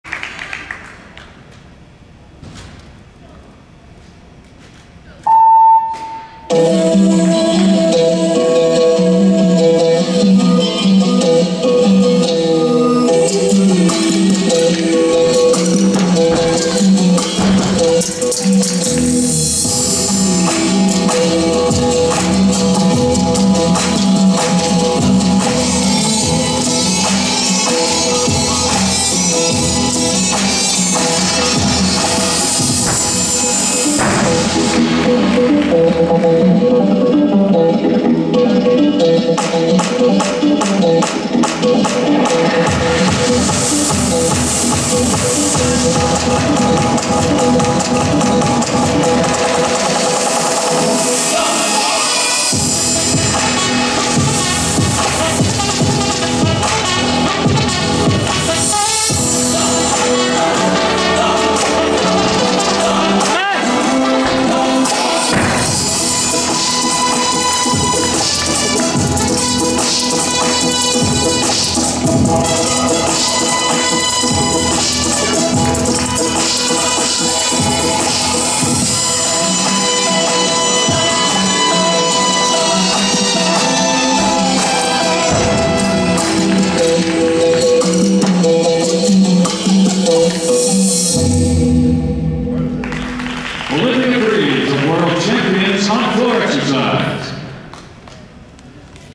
Olivia Greaves – Floor Exercise – 2021 Winter Cup - Senior Women
Feb, 27, 2021 - Indiana Convention Center - Indianapolis, Ind.